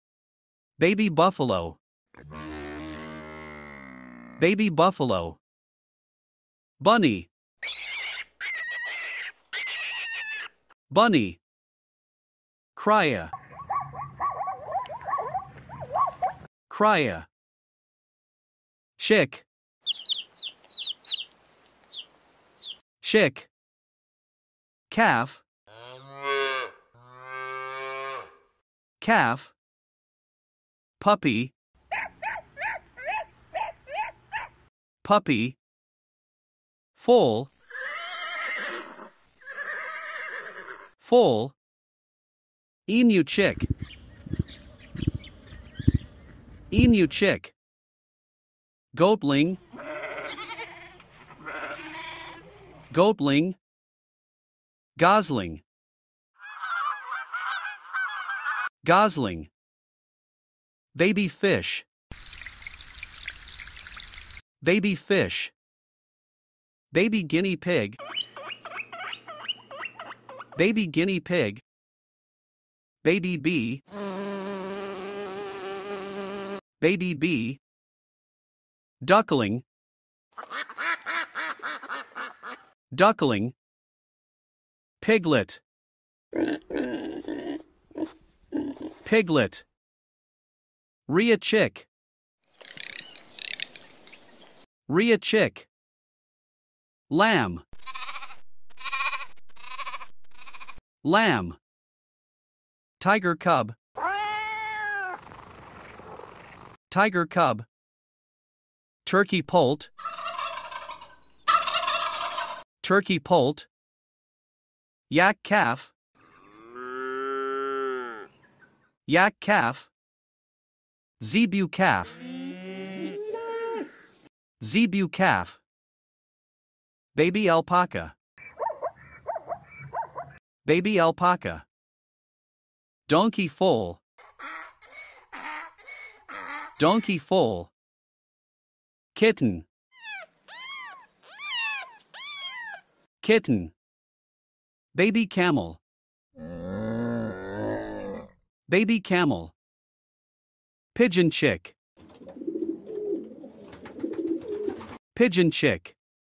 Find out baby farm animals and enjoy their sounds
00:00 Baby Buffalo
00:06 Bunny
00:12 Cria
00:18 Chick
00:24 Calf
00:30 Puppy
00:36 Foal
00:42 Emu Chick
00:48 Goatling
00:54 Gosling
01:00 Baby Fish
01:06 Baby Guinea Pig
01:12 Baby Bee
01:18 Duckling
01:24 Piglet
01:30 Rhea Chick
01:36 lamb
01:42 Tiger Cub
01:48 Turkey Poult
01:54 Yak Calf
02:00 Zebu Calf
02:06 Baby Alpaca
02:12 Donkey Foal
02:18 Kitten
02:24 Baby Camel
02:30 Pigeon Chick